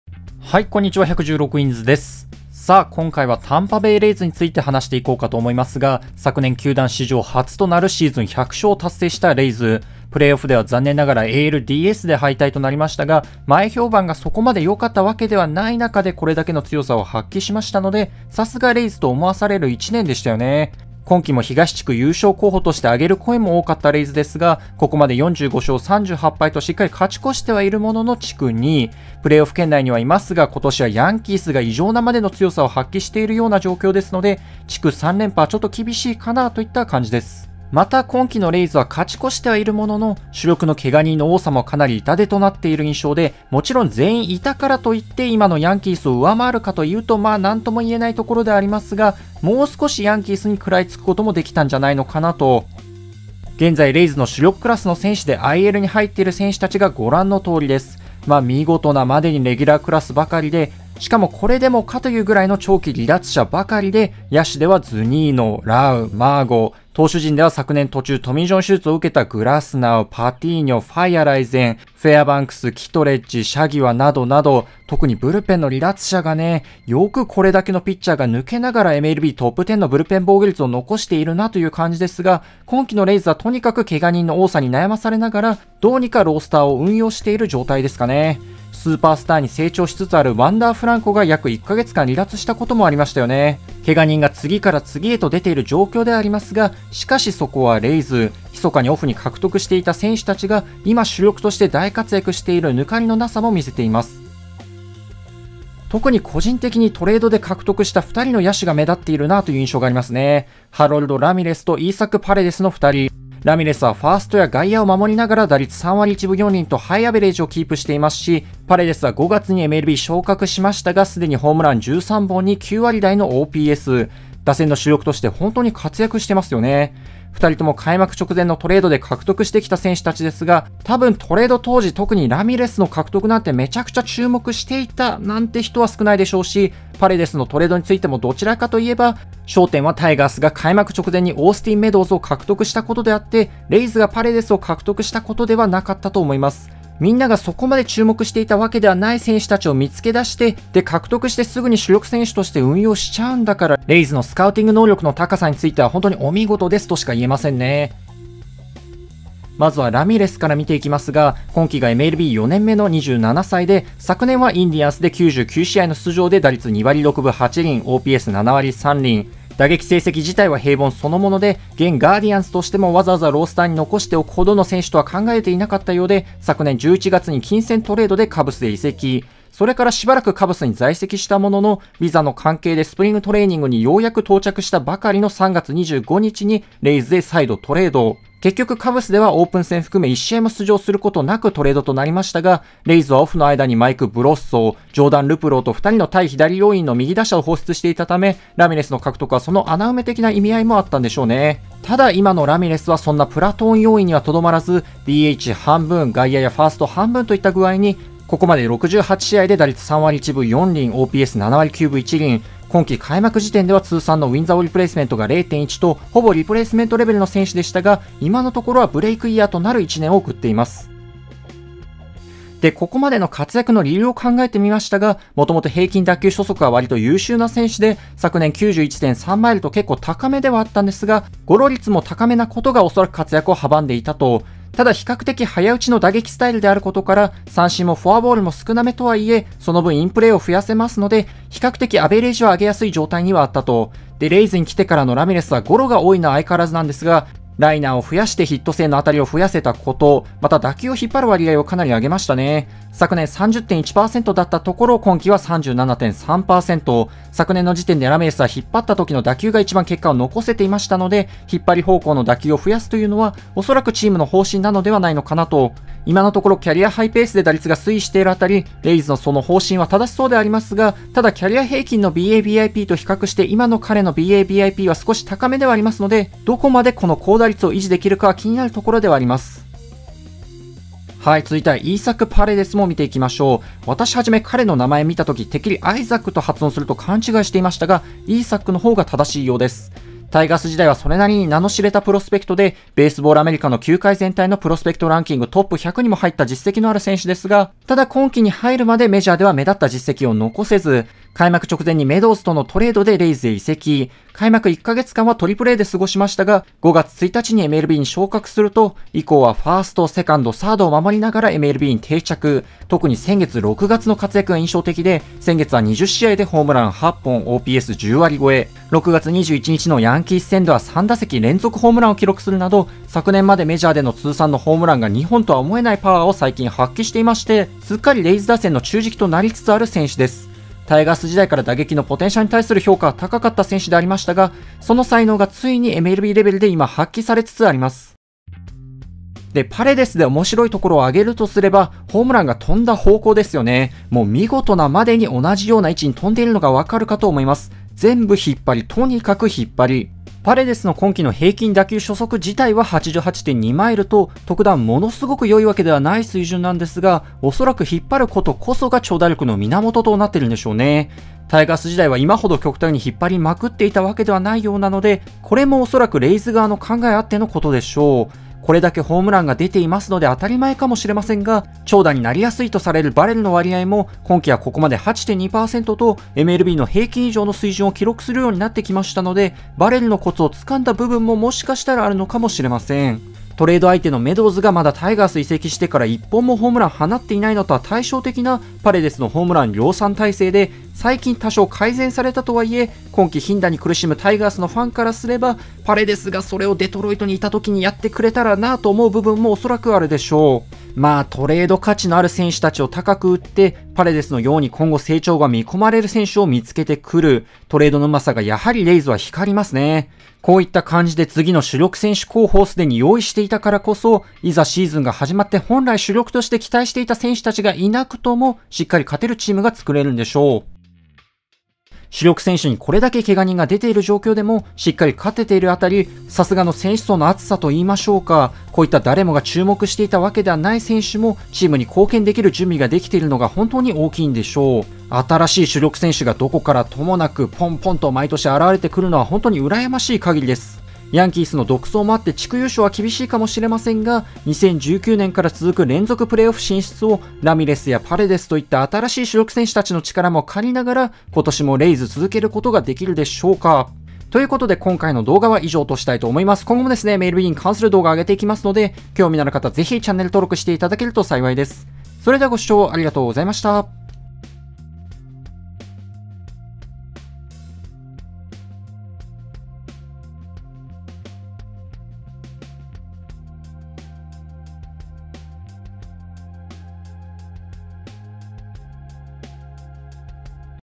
【MLB】敏腕球団レイズさん、またも主力級野手を見つけてくる。
これだけぽんぽん主力選手を見つけてこられるのは本当に羨ましい限り。